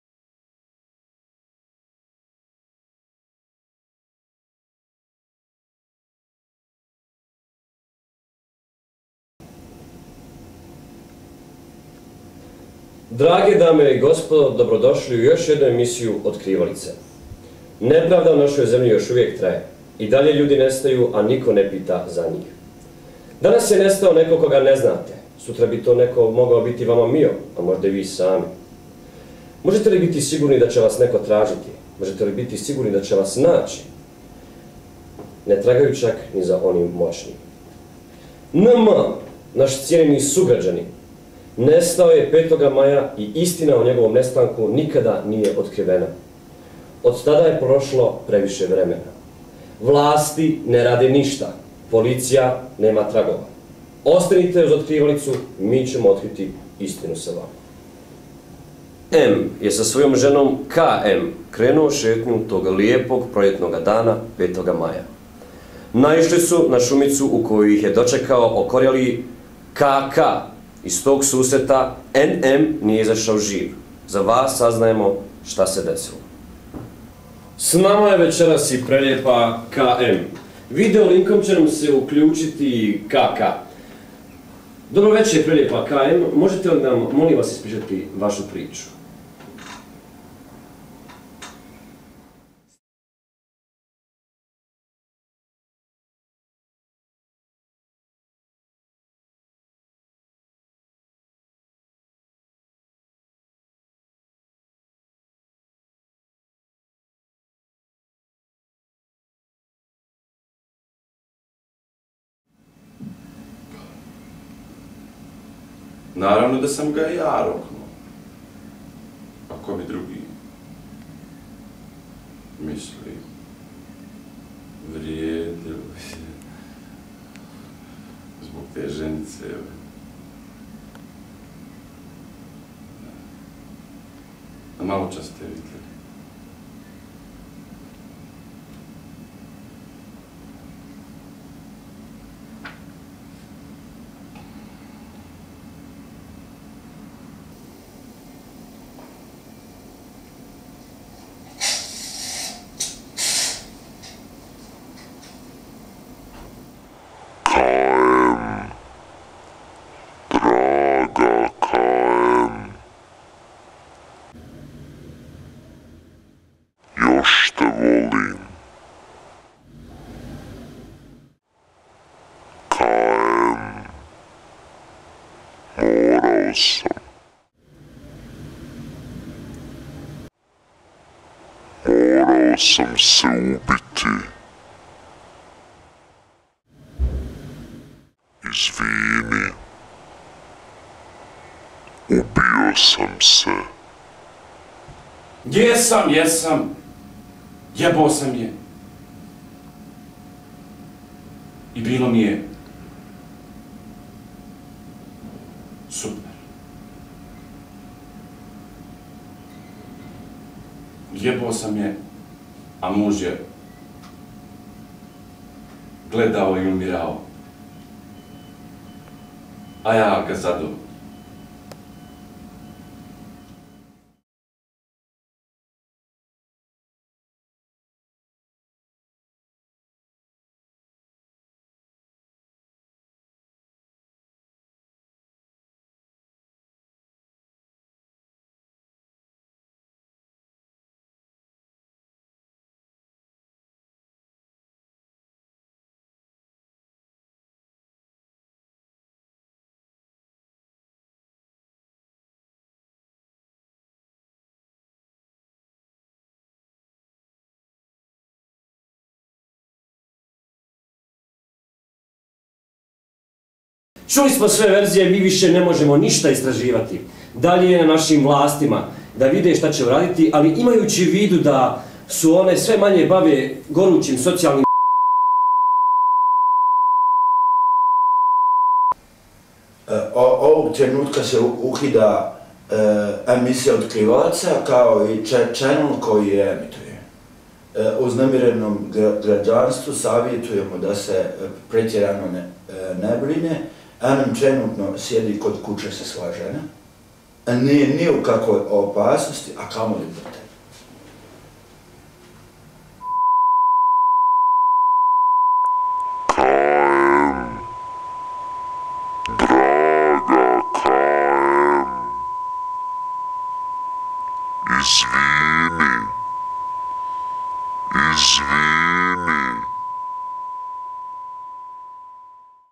Rashomon, citat
Studentska vjezba "Citat" iz predmeta Elementi filma i razvoj filmskih vrsta
Scenarij i rezija: Tina Smalcelj
kamera: Faruk Loncarevic
igraju: Vedran Dekic, Aida Bukva, Muhamed Hadzovic, Narcis Babic